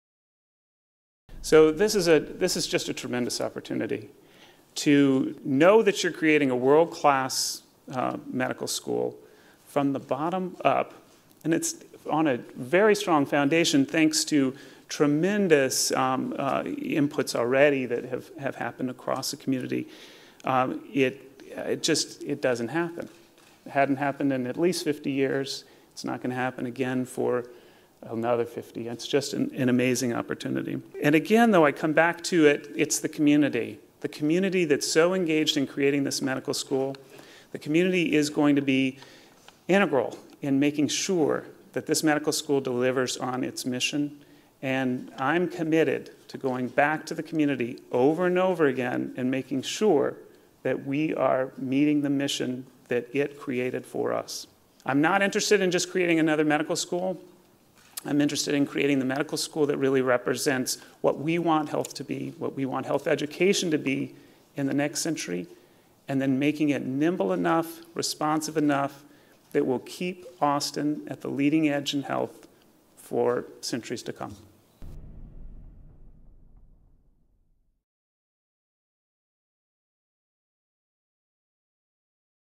Building a Medical School … From the Ground Up
Dr. Clay Johnston, inaugural dean of the Dell Medical School at The University of Texas at Austin, lays out his vision for creating a world-class medical school and what it will mean for the community. (Video from press conference introducing Dr. Johnston as Dean: January 21, 2014)